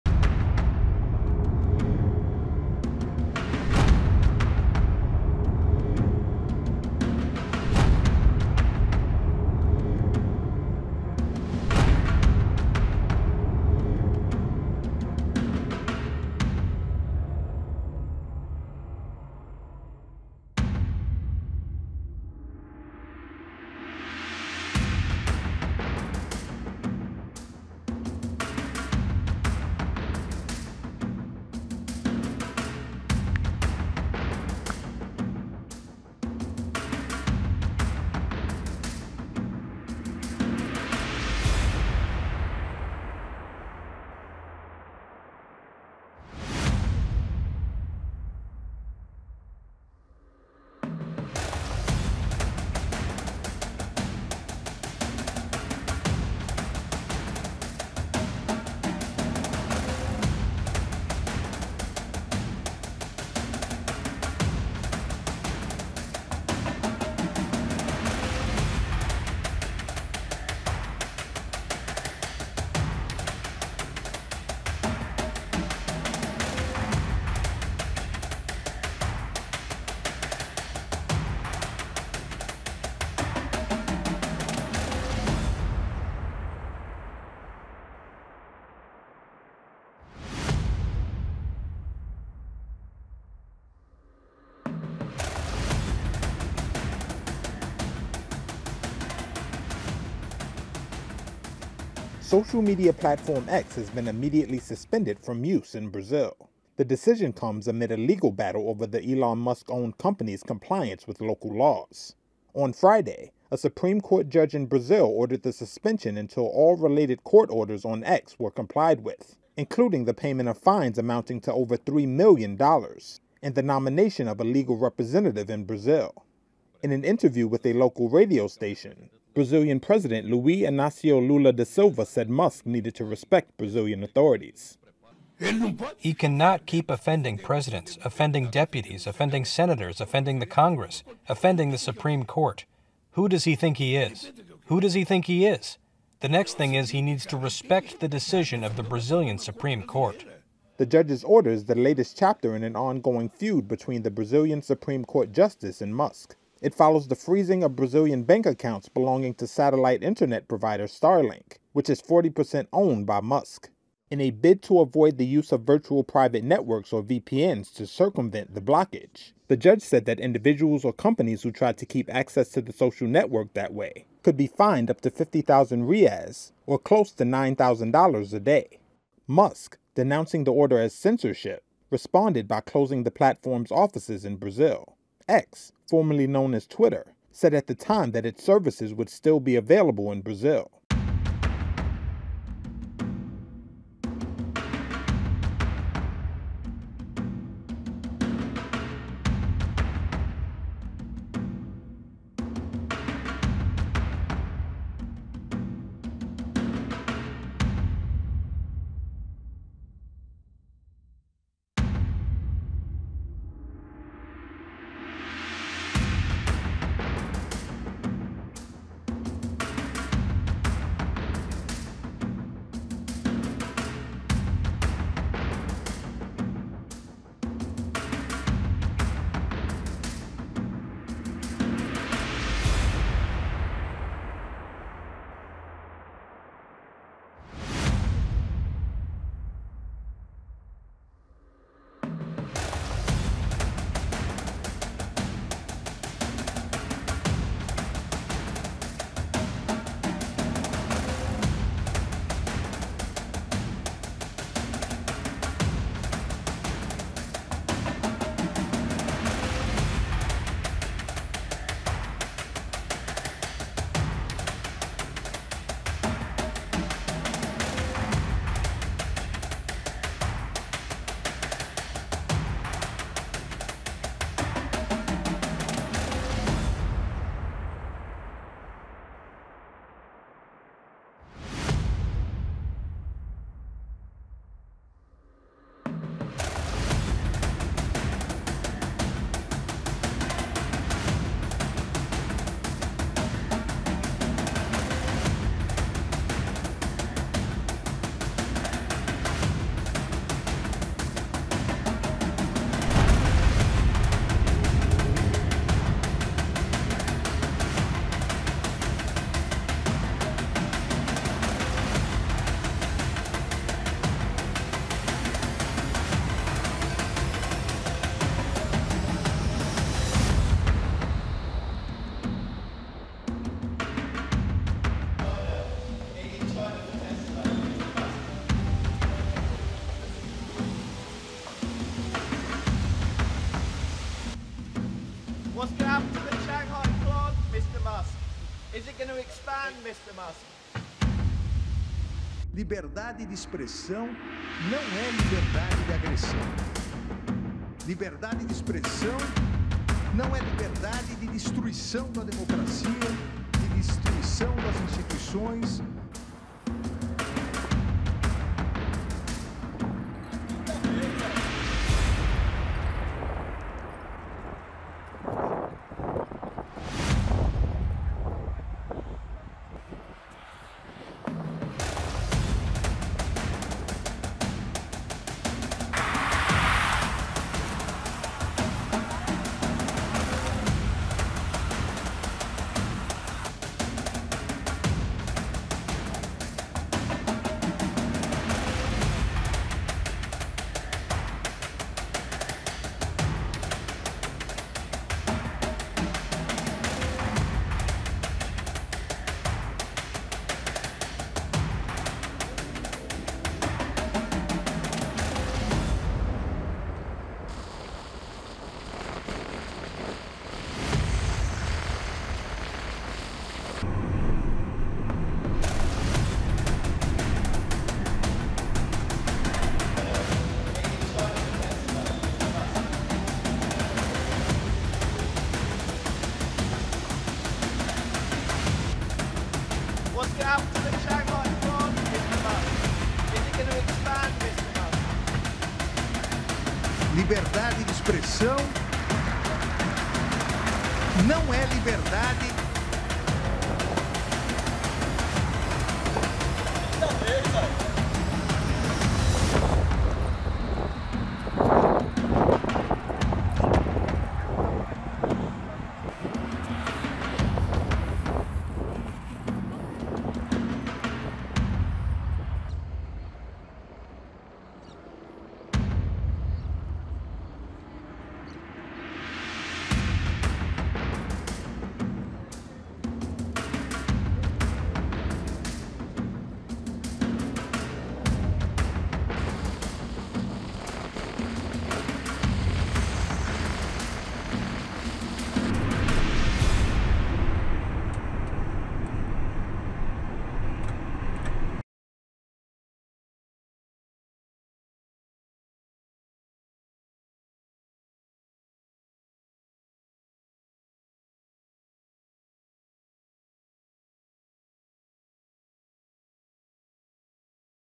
X Goes Offline in Brazil After Supreme Court's Order | Millions Seek Alternatives | N18G
On September 1, Brazil witnessed the shutdown of one of the most popular social networking platforms, X (formerly Twitter), following a Supreme Court order. The platform went offline after Elon Musk failed to comply with local laws by not appointing a new legal representative in an ongoing case. Millions of Brazilian users were left searching for alternatives as internet providers and mobile companies enforced the court's directive.